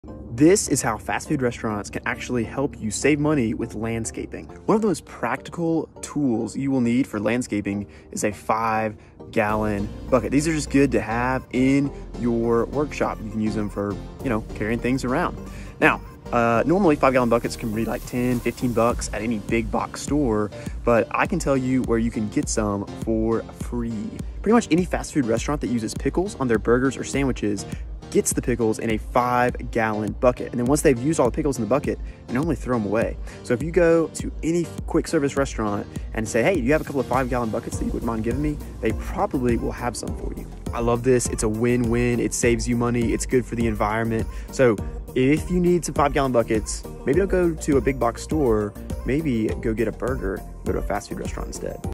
FASTFOOD AND LANDSCAPING? | This tip will save you MONEY
Any fast food restaurant that has pickles on the menu will probably have a ton of 5-gallon buckets that they throw away. Be eco friendly and budget friendly, by asking for some of these buckets instead of purchasing them at a big box store.